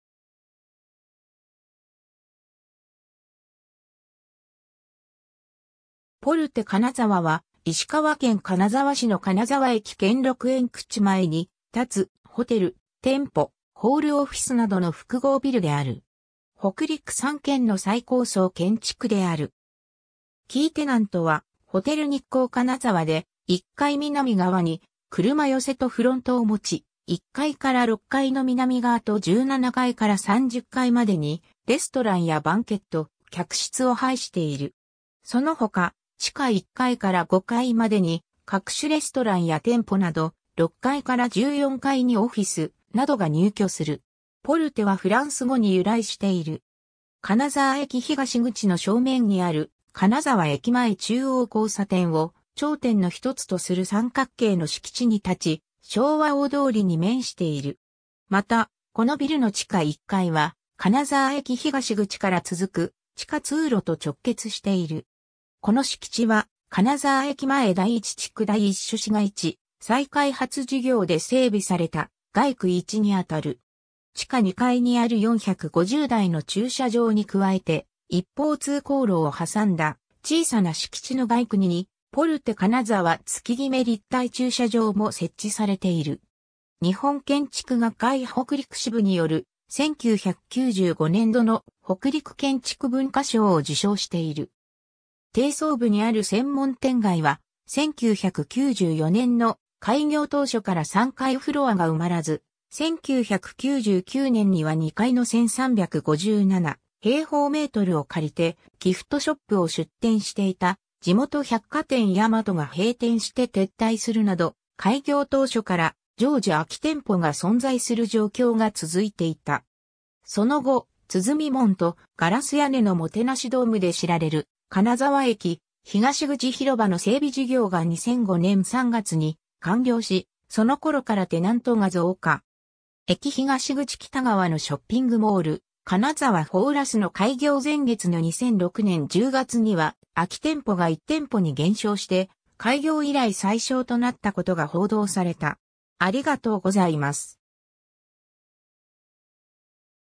ポルテ金沢
ポルテ金沢（ポルテかなざわ）は、石川県金沢市の金沢駅兼六園口（東口）前に建つ、ホテル・店舗・ホール・オフィスなどの複合ビルである。
 北陸3県の最高層建築である。
キーテナントはホテル日航金沢で、1階南側に車寄せとフロントをもち、1階から6階の南側と17階から30階までに、レストランやバンケット、客室を配している。
そのほか、地下1階から5階までに各種レストランや店舗など、6階から14階にオフィスなどが入居する。
「ポルテ」はフランス語に由来している。
 金沢駅東口の正面にある金沢駅前中央交差点を頂点の1つとする三角形の敷地に建ち、昭和大通り（石川県道146号金沢停車場南線）に面している。
また、このビルの地下1階は、金沢駅東口から続く地下通路と直結している。
 この敷地は、金沢駅前第一地区第一種市街地再開発事業で整備された街区1に当たる。
地下2階にある450台の駐車場に加えて、一方通行路を挟んだ小さな敷地の街区2に、ポルテ金沢月極立体駐車場も設置されている。
 日本建築学会北陸支部による、1995年（平成7年）度の北陸建築文化賞を受賞している。
 低層部にある専門店街は、1994年（平成6年）の開業当初から3階フロアが埋まらず、1999年（平成11年）には2階の1,357m2を借りてギフトショップを出店していた地元百貨店大和が閉店して撤退するなど、開業当初から常時空き店舗が存在する状況が続いていた。
 その後、鼓門（つづみもん）と、ガラス屋根のもてなしドームで知られる、金沢駅東口広場の整備事業が2005年（平成17年）3月に完了し、その頃からテナントが増加。
駅東口北側のショッピングモール、金沢フォーラスの開業前月の2006年（平成18年）10月には、空き店舗が1店舗に減少して、開業以来最小となったことが報道された。